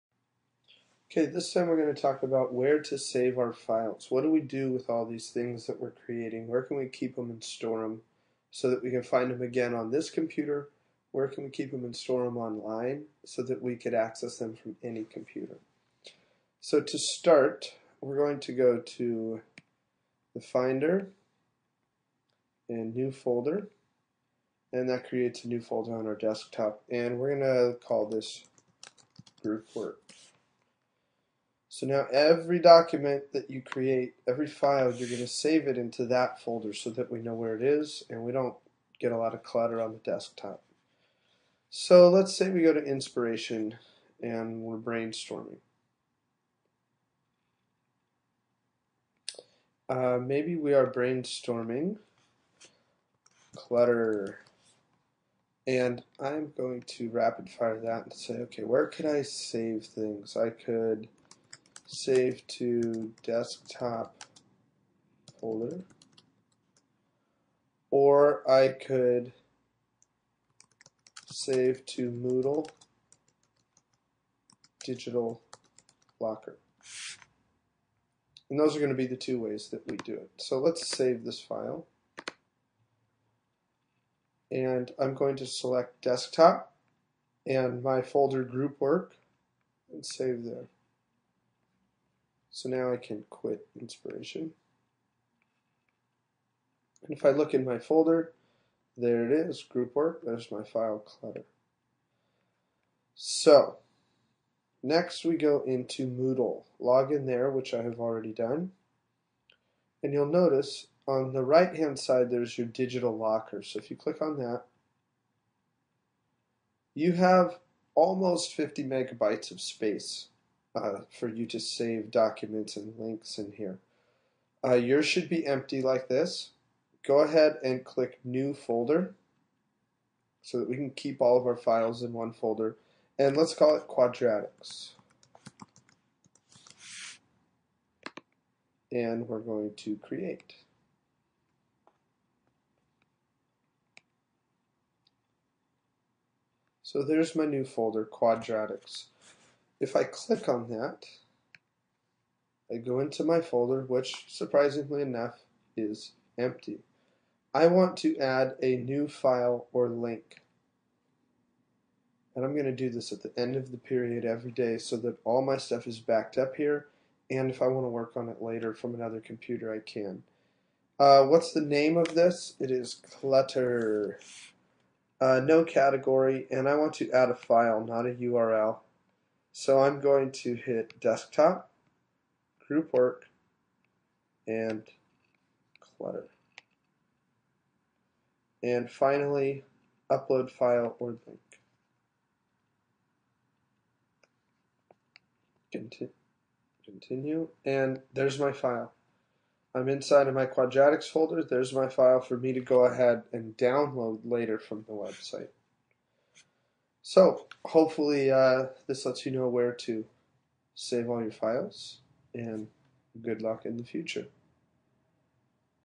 Organizing Files
Where to organize your files for the Quadratics Project on your desktop and in your Moodle digital locker.